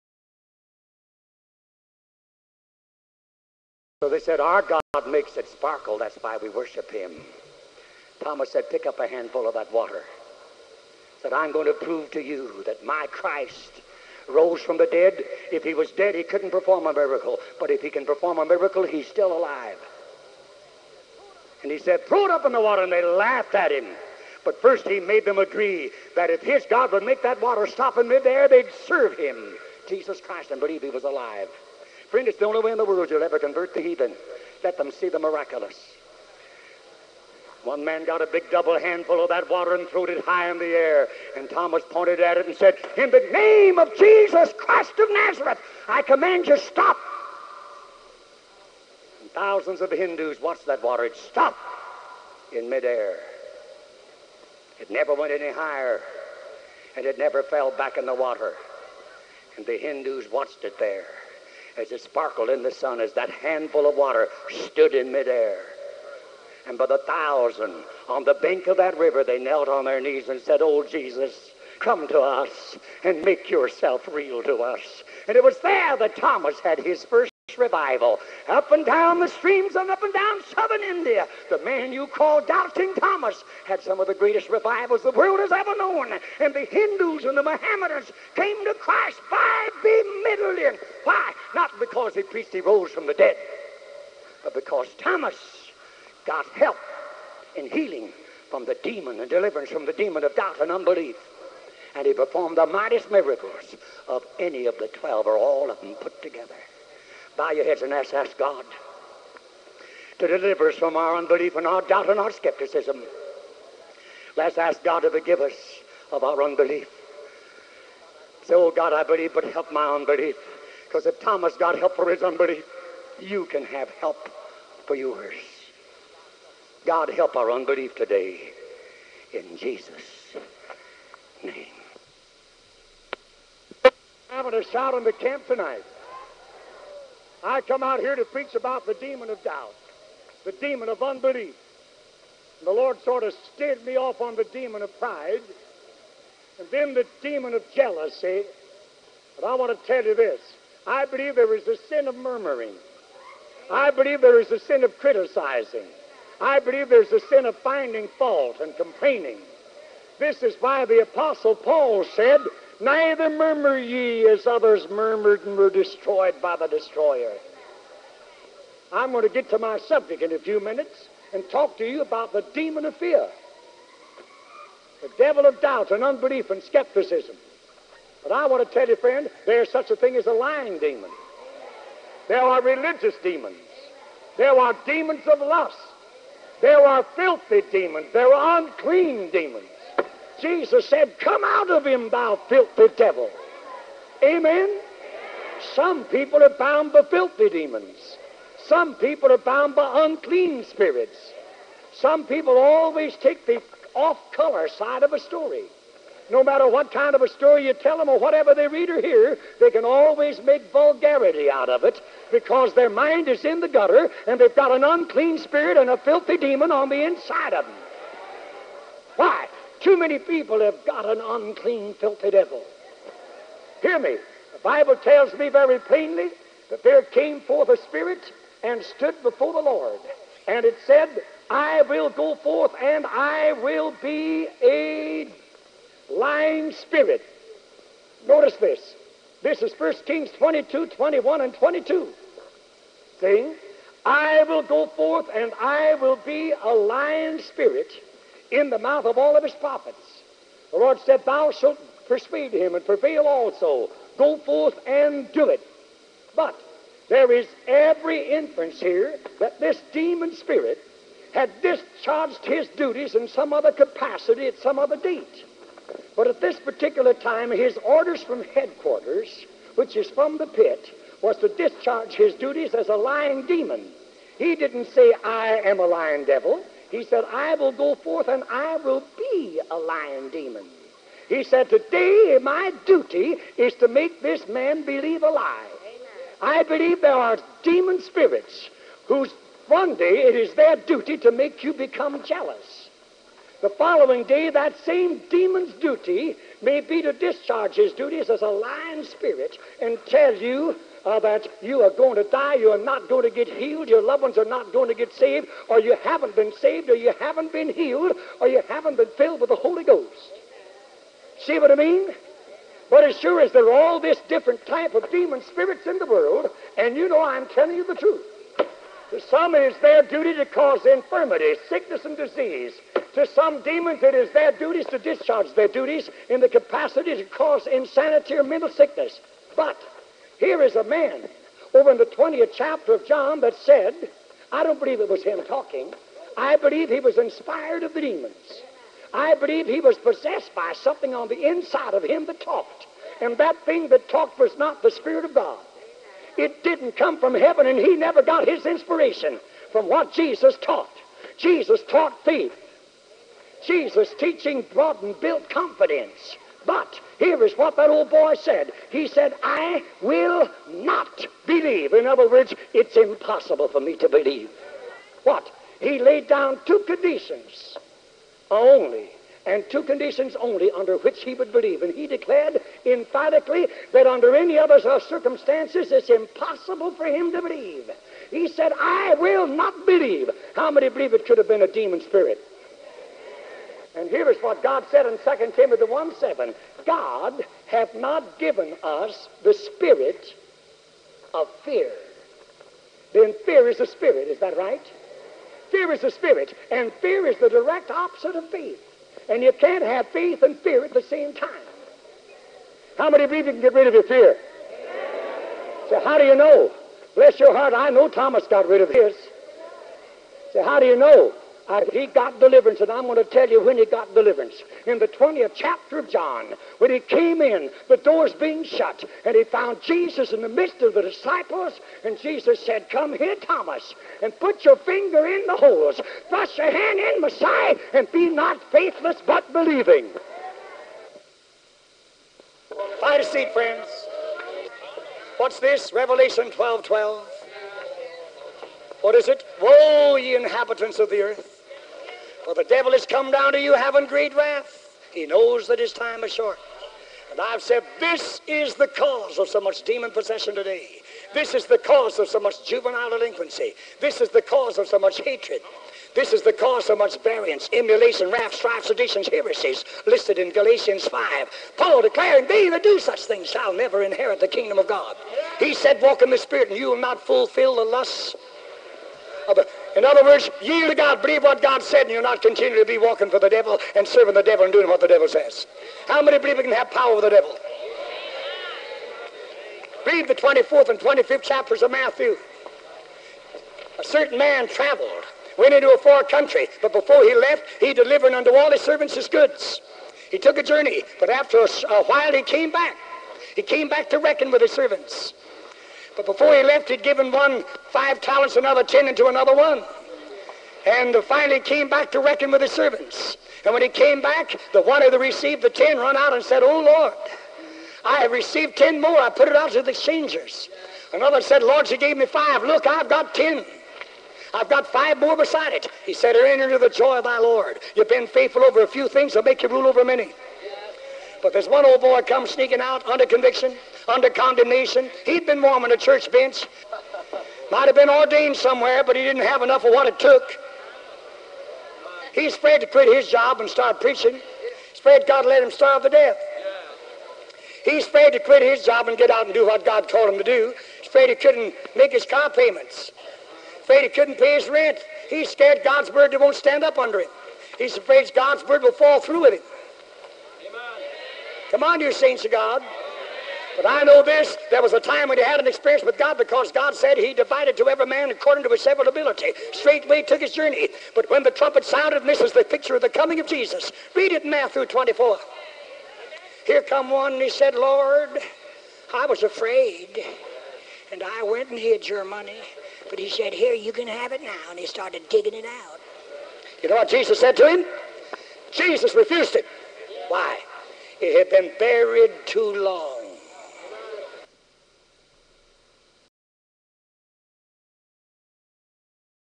FEAR, UNBELIEF, AND DOUBT #5
This is a very powerful sermon on FEAR, UNBELIEF, AND DOUBT as it was given by Reverend A. A. Allen